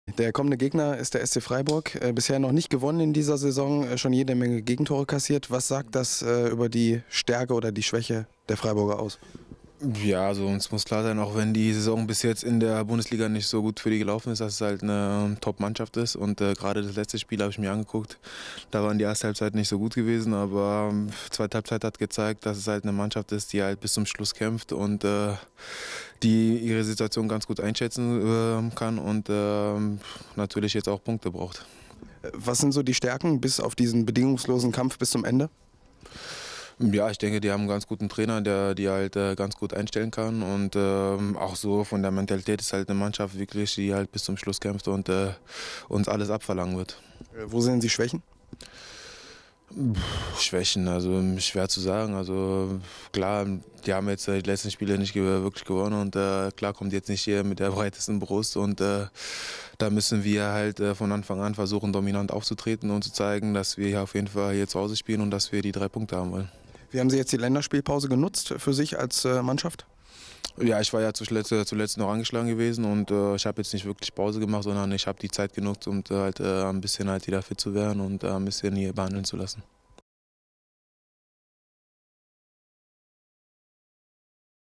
Lukimya spricht über den SC Freiburg
Werders Abwehrspieler Assani Lukimya hat die Länderspiel-Pause genutzt, um sich nach seiner Verletzung behandeln zu lassen und fit zu werden. Im Interview spricht Lukimya außerdem über Stärken und Schwächen des SC Freiburg.